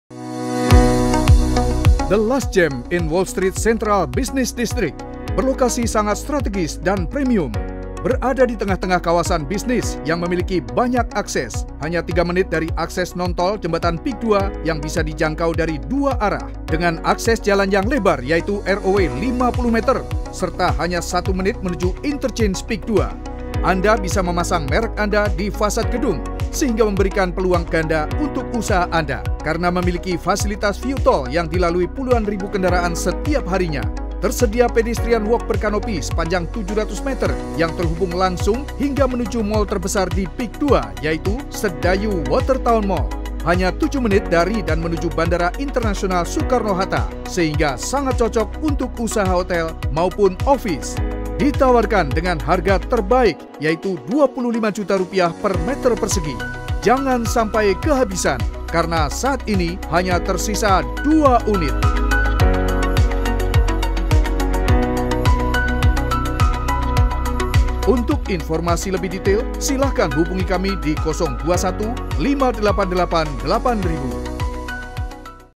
Dari Ratusan, Kini Tersisa 2 Unit!
Hallo Sobat PIK2!

Kawasan komersial ekslusif yang berlokasi sangat strategis, berseberangan dengan akses toll.

Kawasan yang dilengkapi dengan mall terbesar di PIK2 yaitu Sedayu Watertown Mall, yang menghadirkan berbagai macam fasilitas serta tenant-tenant ternama.

Kawasan ini akan menjadi pusat kota PIK2, karena selain mall terbesar, juga akan dikelilingi oleh hotel-hotel Bintang 5 ternama.

Serta kawasan bisnis kelas dunia seperti, Islamic Financial Center, Manhattan Avenue, dll yang semuanya akan terhubung dengan fasilitas Skybridge langsung, sehingga sangat nyaman berjalan kaki sambil menikmati fasilitas dikawasan ini.

PIK2 sungguh akan menjadi satu kota baru dengan potensi kawasan yang menjanjikan, dengan hadirnya kawasan-kawasan eksklusif tersebut

Info Properti PIK2, hub:
Kunjungi Marketing Gallery PIK2:
Jl. Marina Indah Raya No.1 Pantai Indah Kapuk – Jakarta Utara.